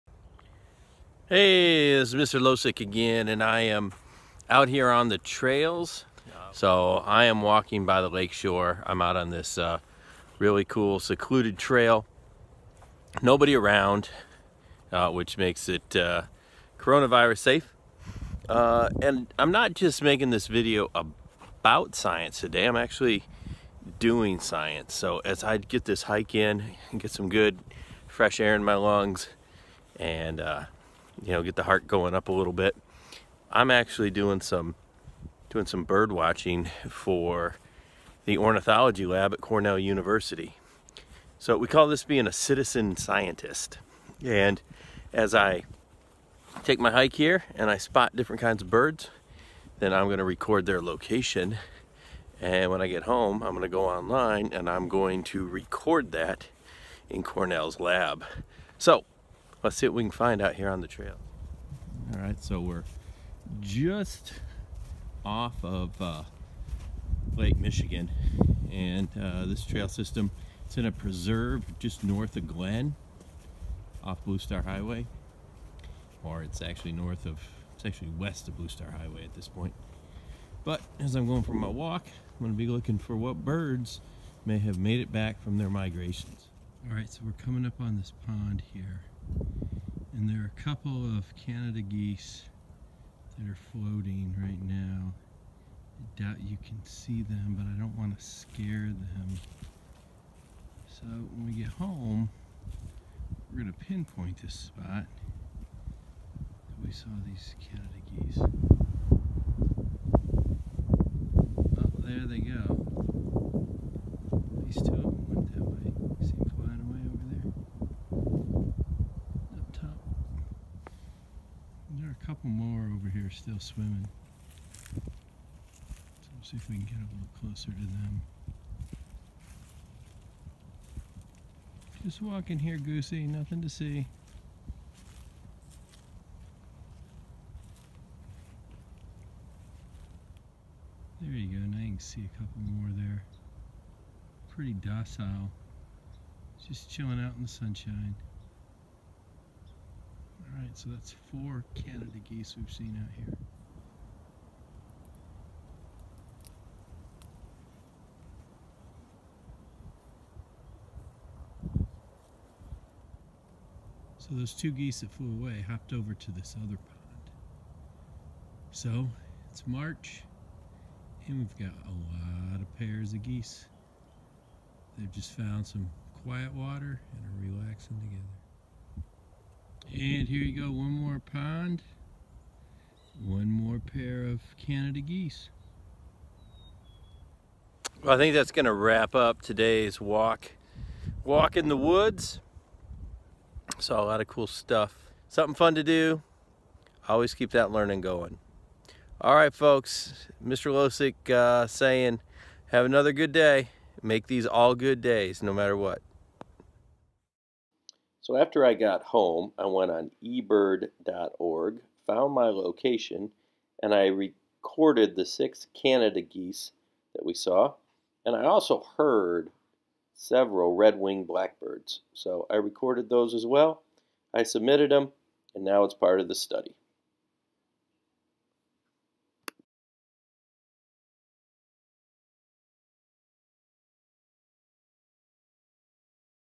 In the Woods with Mr. Losik: Be a Citizen Scientist
Don't just learn about science, do science. eBird.org is a great place to record birds you see in and around your area.